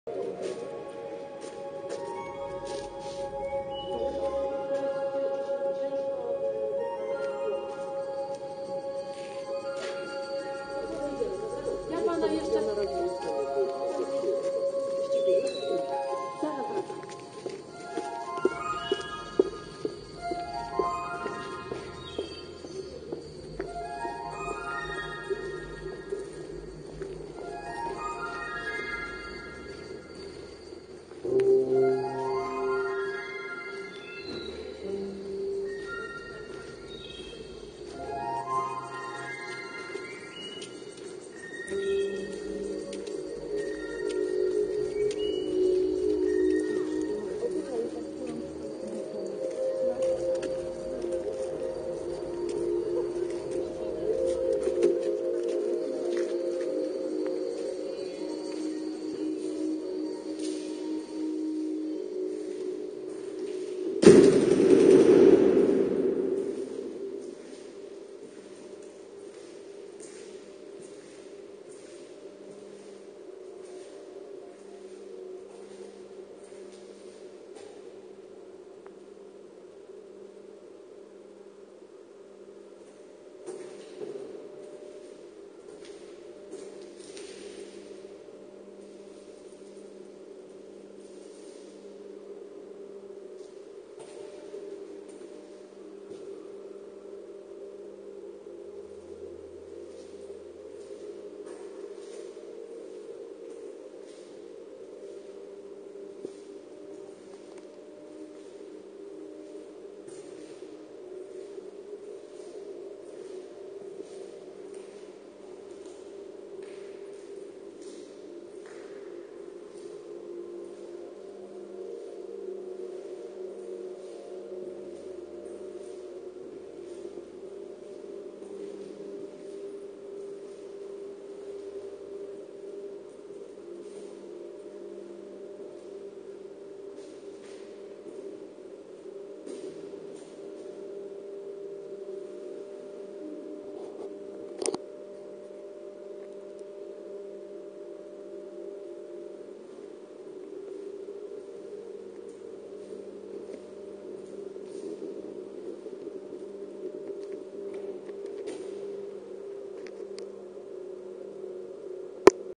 2014.11.21 19:43, MetaMemory, 19 x 19, CLIP0263
Wernisaż "19 x 19" - wystawy 19 artystów w 19. dzielnicy w Warszawie, ul. Kolejowa 45;